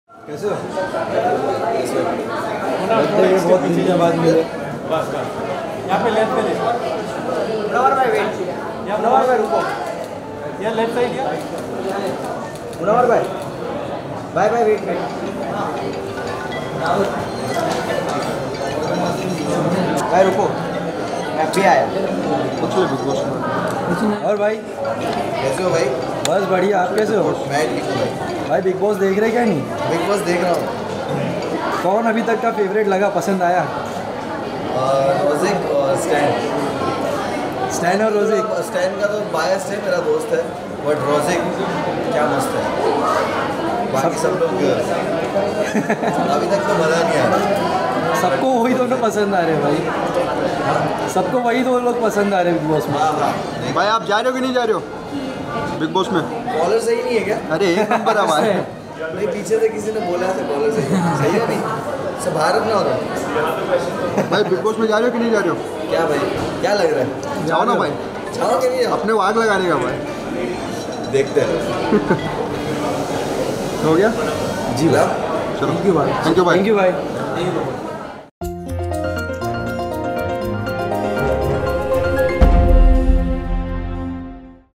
Bigg Boss 16 में कौन पसंद आ रहा है Munawar Faruqui को, क्या खुद जाएंगे शो में ?| Lehren Small Screen
रियलिटी शो 'लॉकअप' के विनर रह चुके मुनव्वर फारुकी ने 'बिग बॉस' को लेकर अपना रिएक्शन दिया। साथ ही कौन सा कंटेस्टेंट उन्हें है पसंद उस बारे में भी बताया।